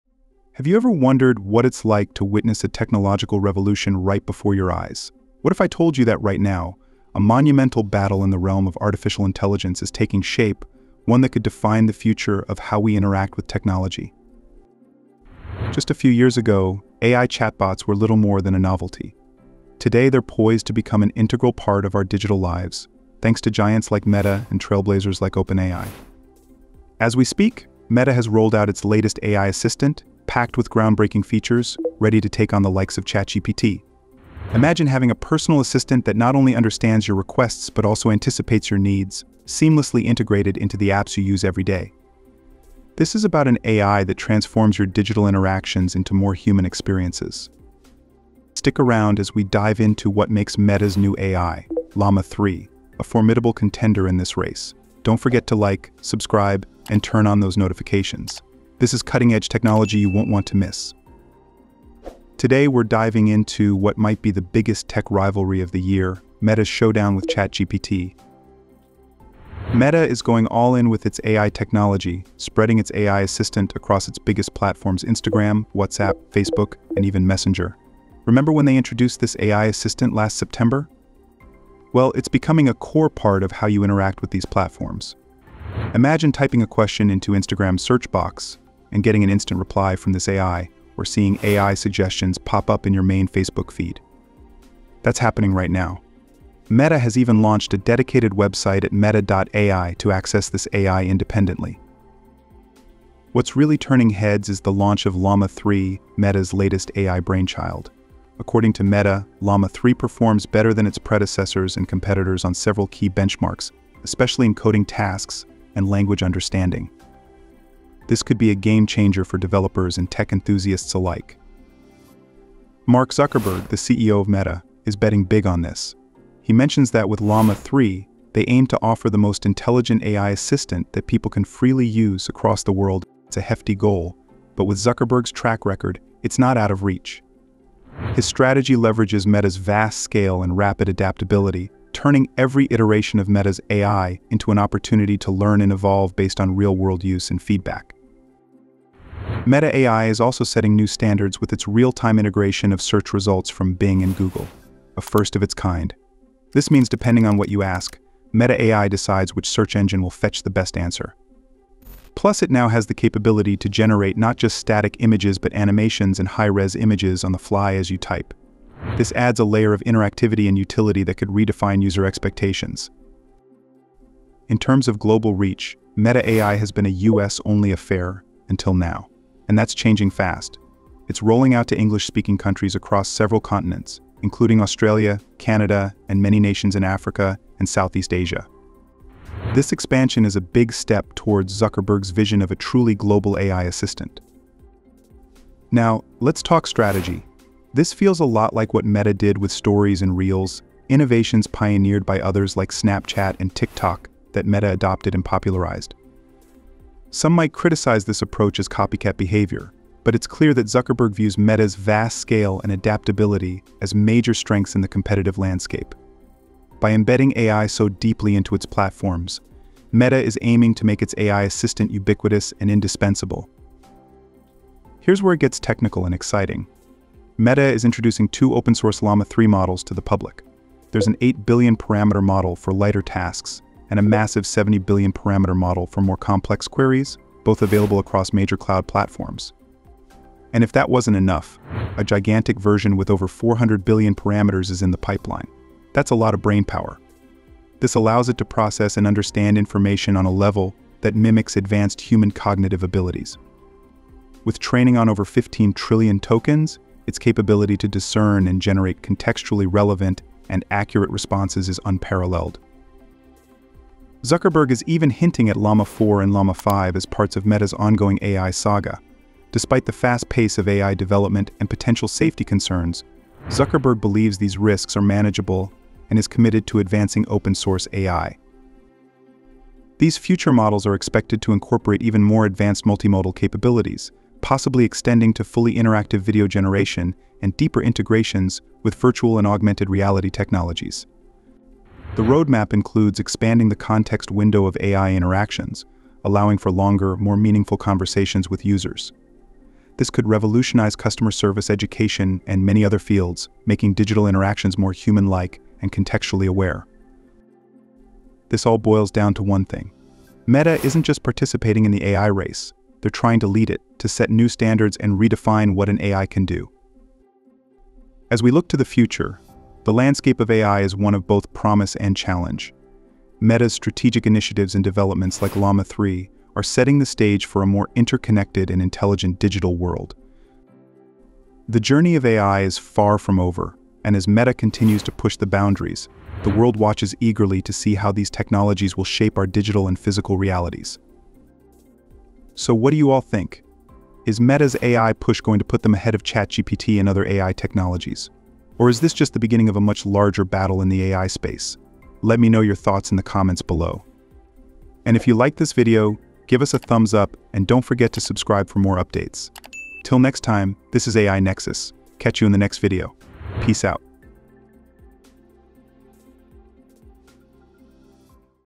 Meta AI With Llama 3 LAUNCHED (Better Than ChatGPT?)
🚀🤖 Witness the Future: Meta vs. ChatGPT in an Epic AI Showdown! Dive into the battle of the titans as Meta rolls out its newest AI, Llama 3, set to redefine our digital lives. 🌐 Discover how Meta's Llama 3, equipped with groundbreaking features, competes against OpenAI's ChatGPT. From social media integrations to innovative coding capabilities, learn how Meta is aiming to lead the AI revolution. 🛠️

CHAPTERS:
0:00 - Introduction
1:04 - Meta AI vs ChatGPT Comparison
5:40 - Viewer Opinions